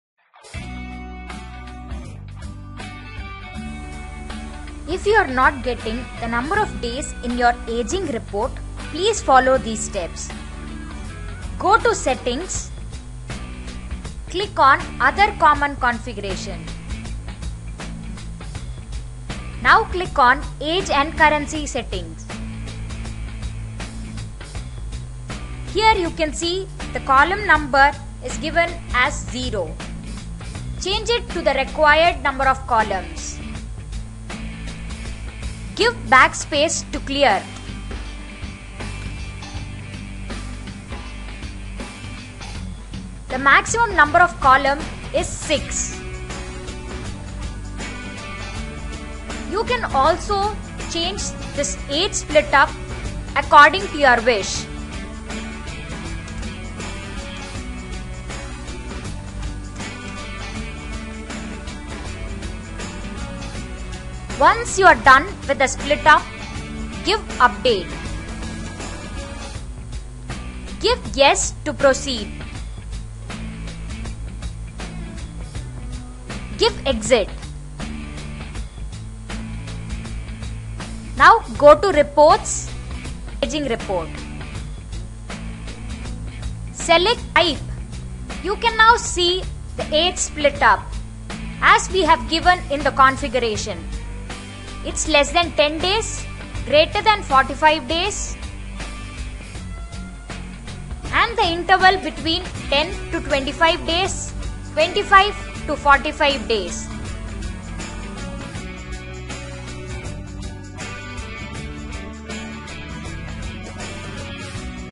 GoFrugal - Ageing Report - TRAC | English
In this video, we will guide you on how to configure Ageing Report and to check Customer and Supplier outstandings agewise.
For any further queries, please raise ticket in myGoFrugal App or come online in Chat.